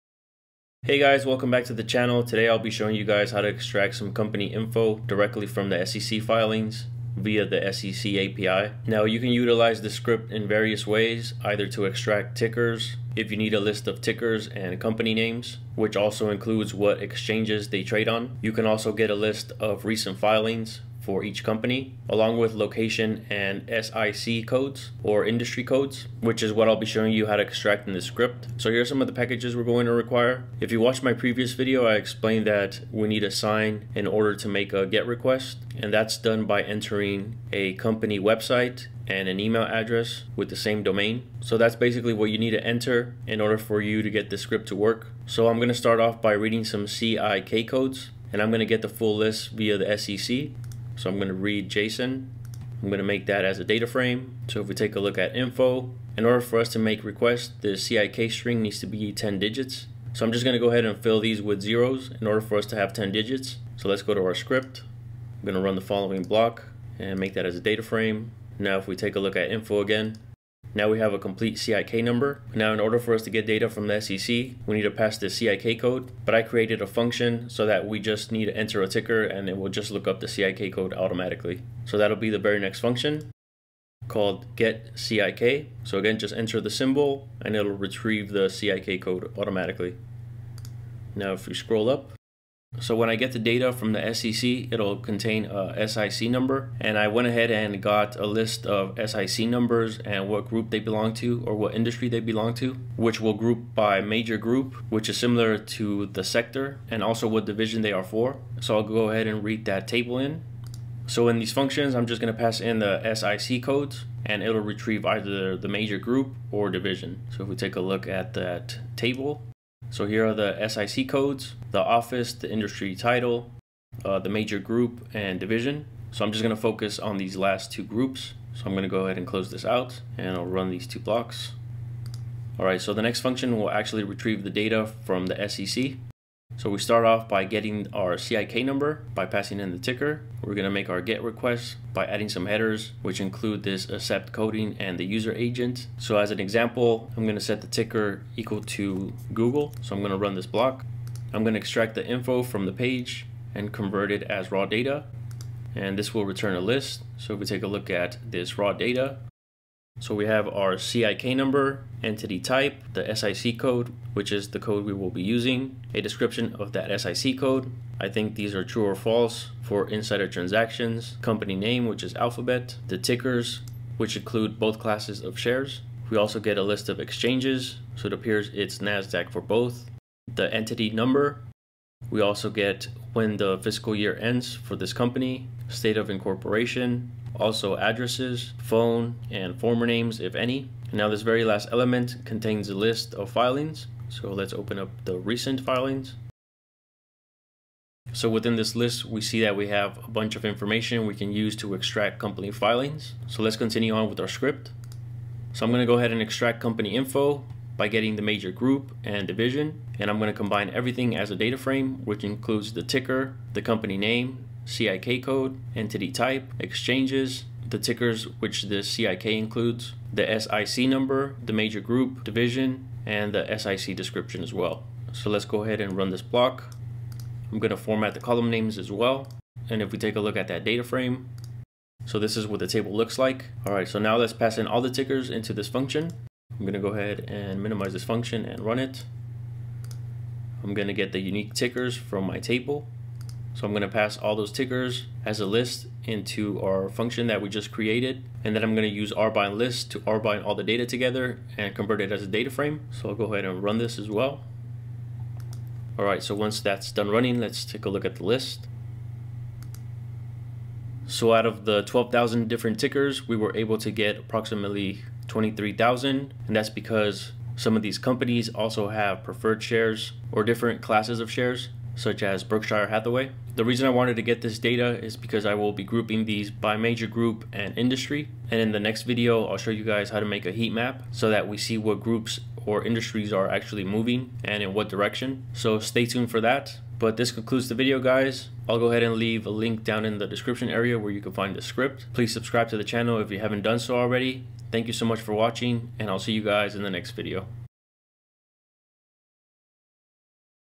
SEC API - Getting Company Information, SIC Codes, Ticker Symbols & Recent Filings | R
Questions?